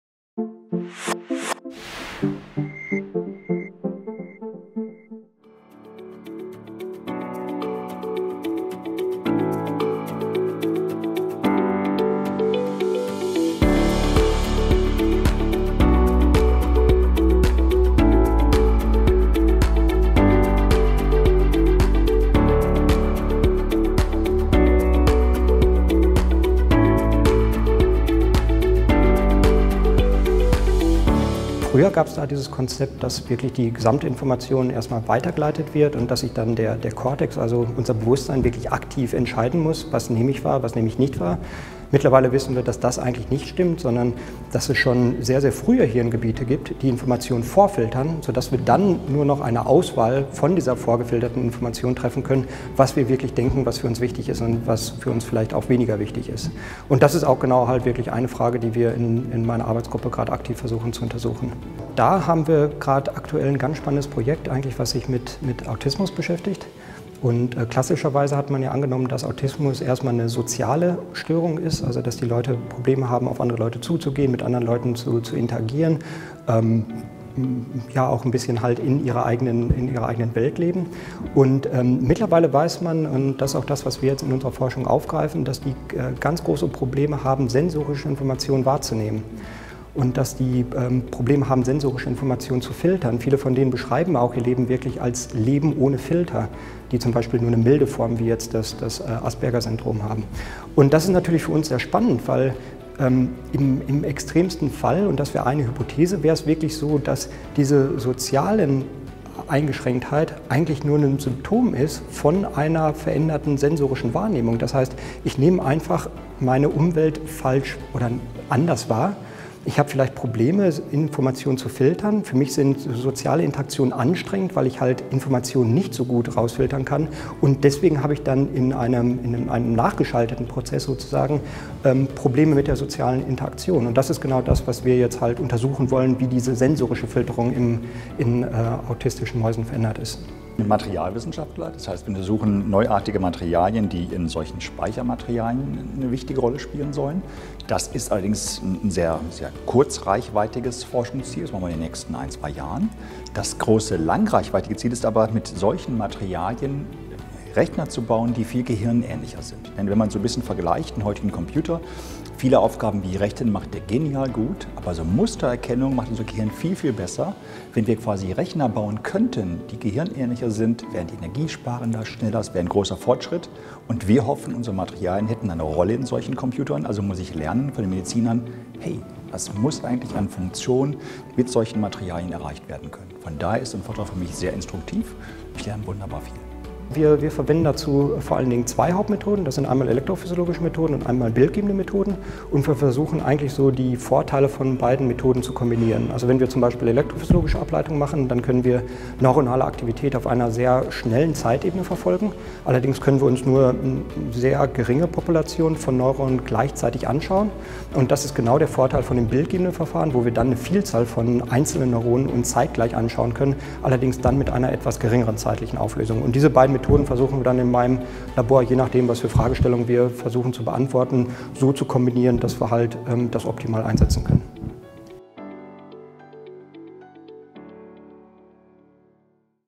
25. Faculty Club-Treffen mit Dr. Markus Rothermel
11. Oktober 2019
„Dem Gehirn beim Arbeiten zusehen”

Unser Gehirn beeinflusst und filtert aktiv die sensorischen Informationen, die unsere Sinnesorgane ständig liefern. Markus Rothermel untersucht in seiner von der DFG geförderten Emmy-Noether-Forschungsgruppe diese Top-Down-Modulation der Informationsverarbeitung. Dabei nutzt er das Riechsystem der Maus als Modell und verwendet eine Vielzahl neuer optogenetischer Methoden in Kombination mit der 2-Photonen-Mikroskopie, um den Neuronen direkt bei der Arbeit zuzusehen.